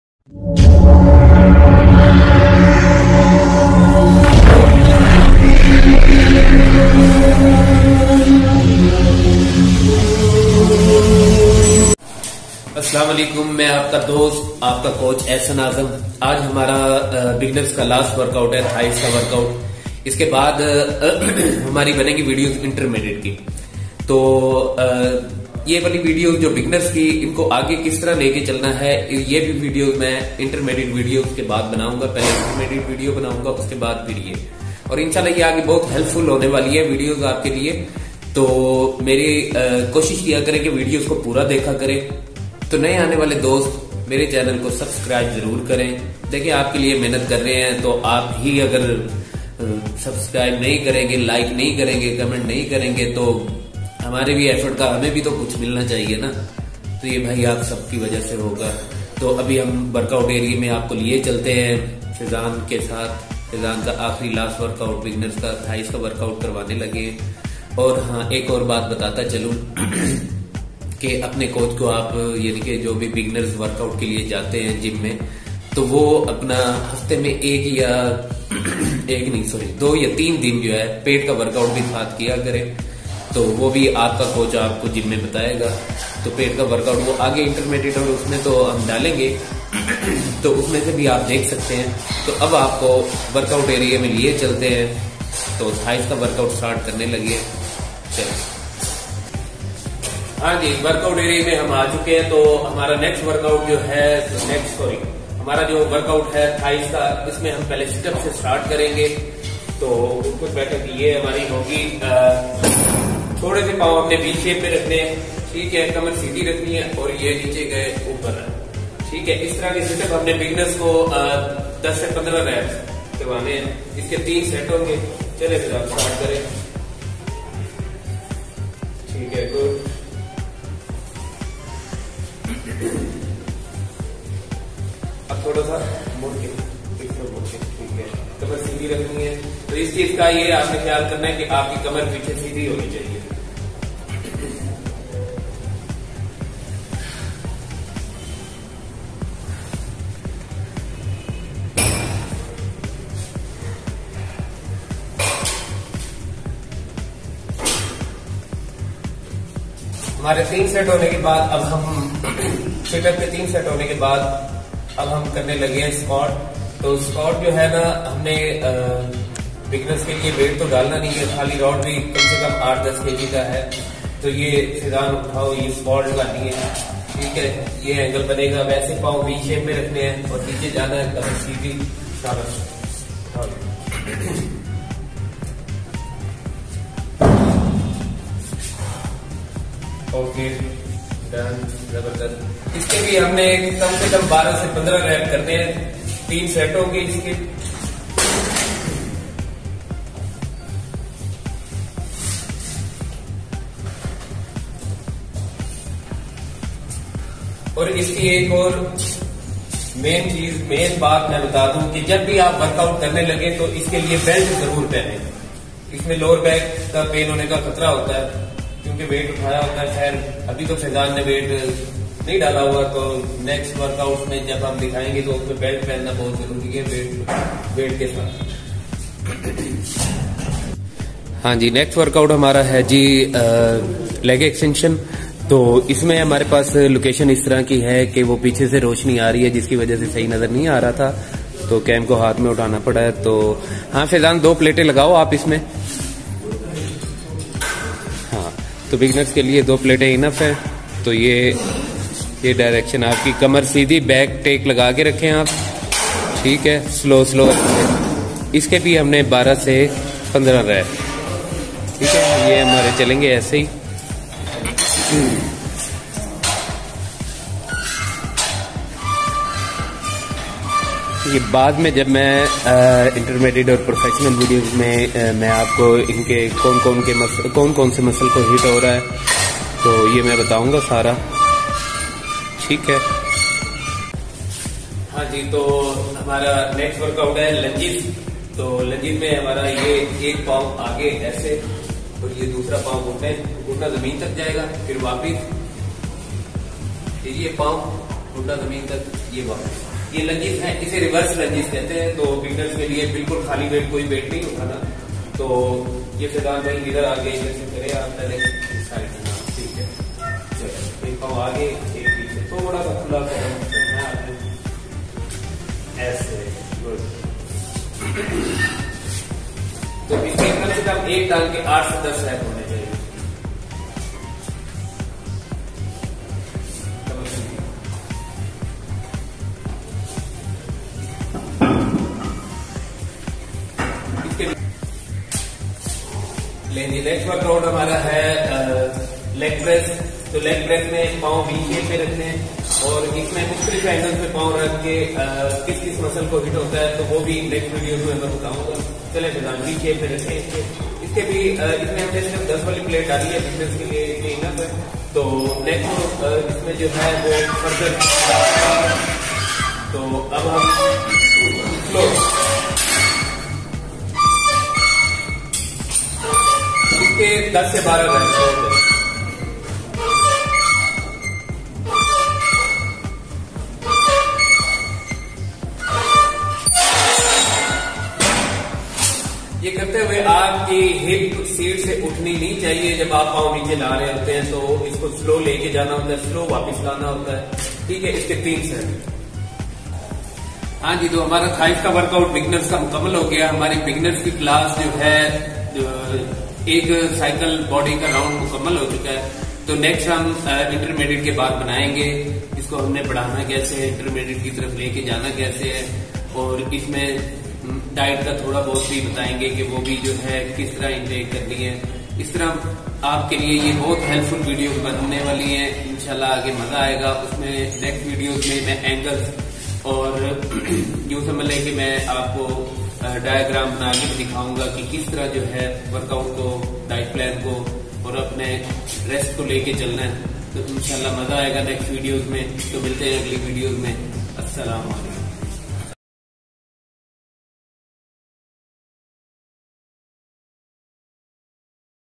GYM! BEGINNER THIGH WORKOUT DAY 6 (2021)
Asalmualikum my name is Ahsan Azam your coach Ur friend today is beginner thigh workout so guys please sport me.